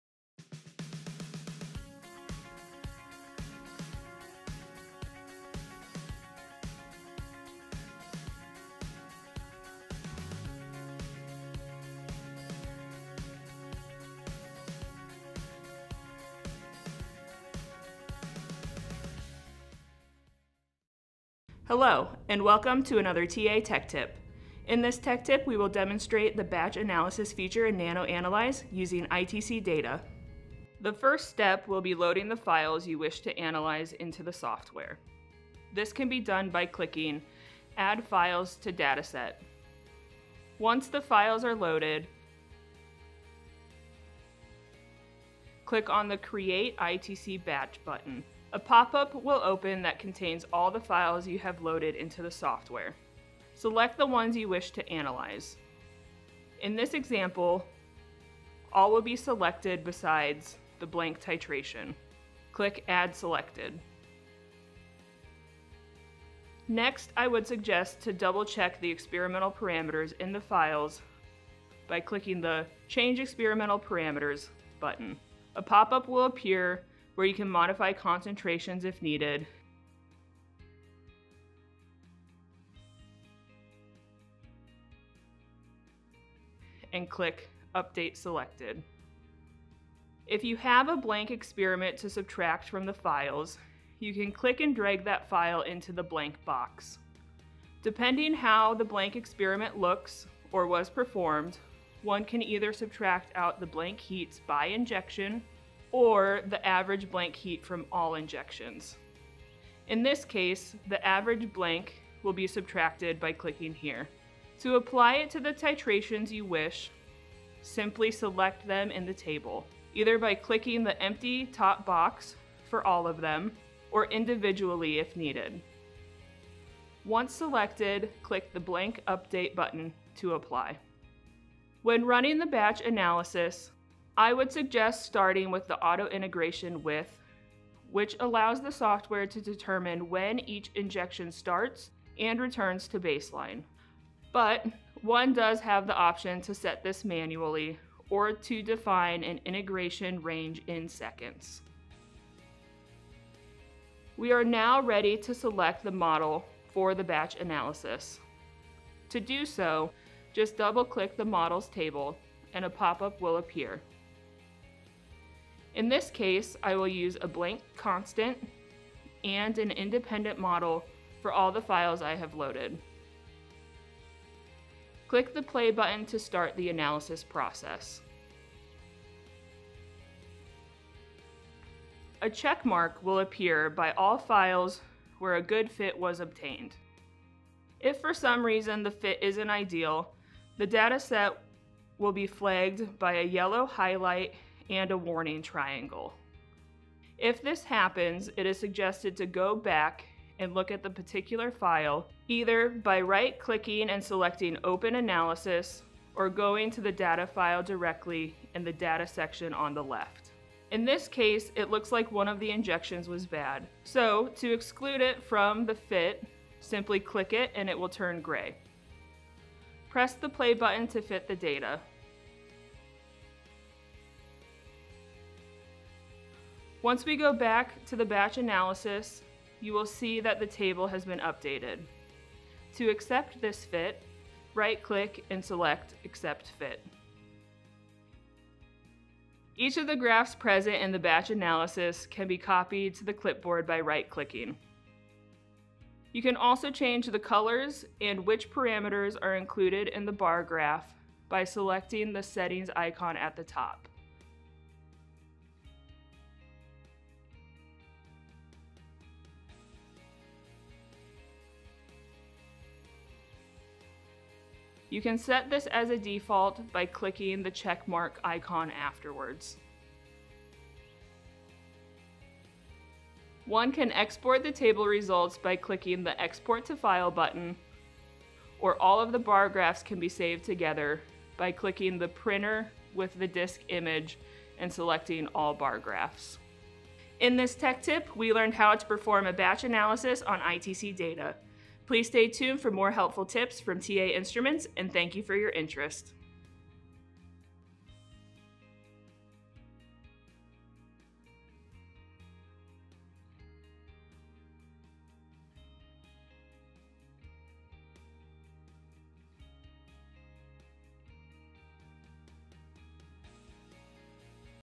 ITC Batch Analysis in NanoAnalyze
In this Tech Tip, we will demonstrate the ITC Batch Analysis in NanoAnalyze.